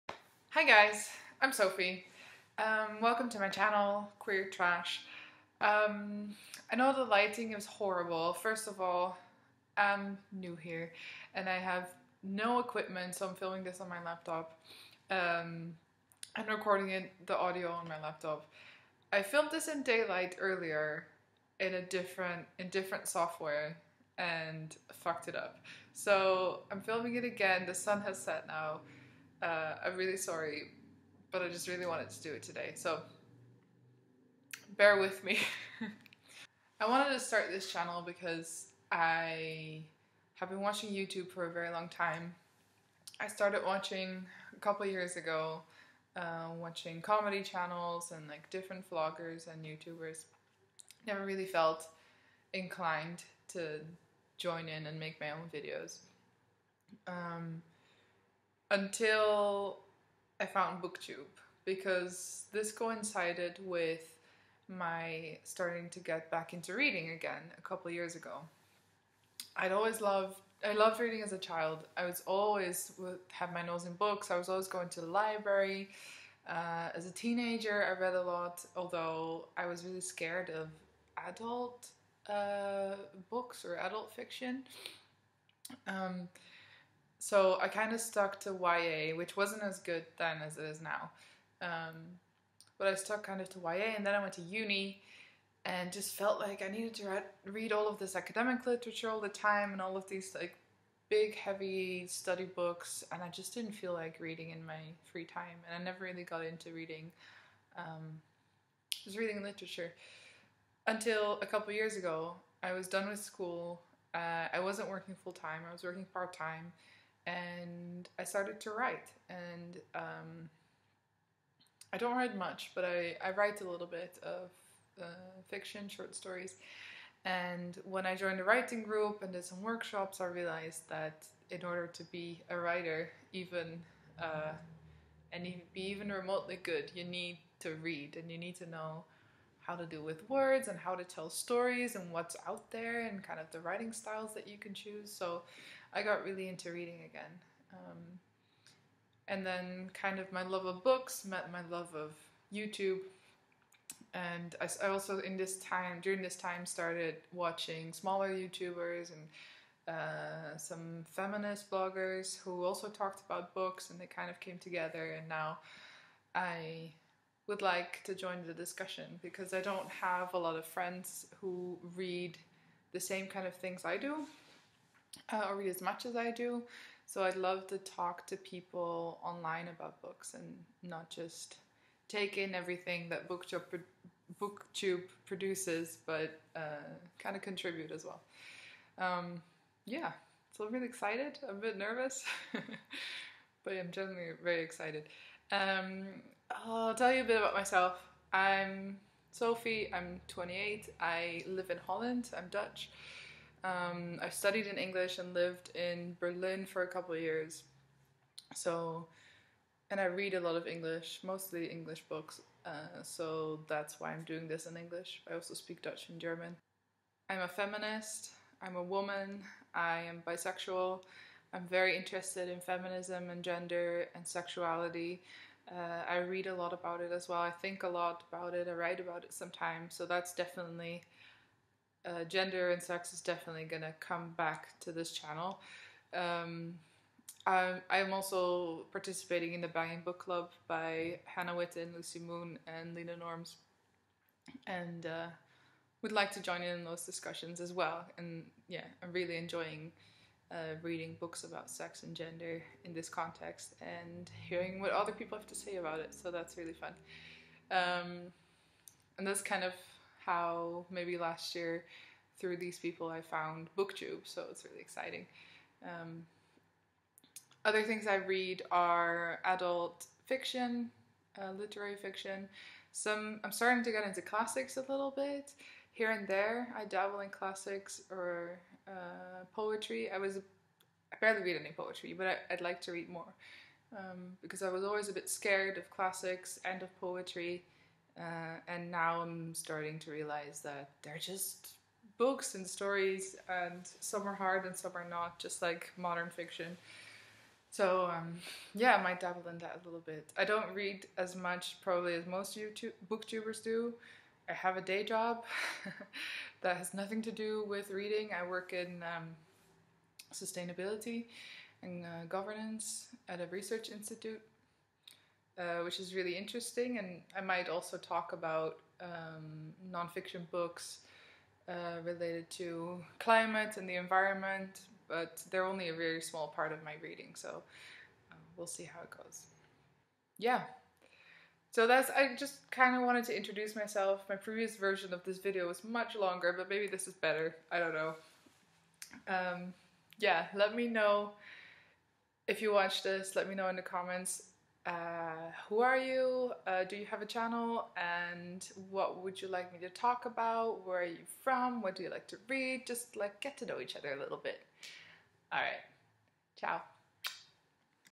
Welcome to my booktube channel! // Queer Trash
Hi guys!

I'm Sophie a.k.a. Queer Trash, and I'm super new here :) I hope you enjoy my first little introduction video. I'll be posting videos about books, feminism, sexuality and just chats - hopefully I'll get better with tech stuff too!

Let me know how you're doing below!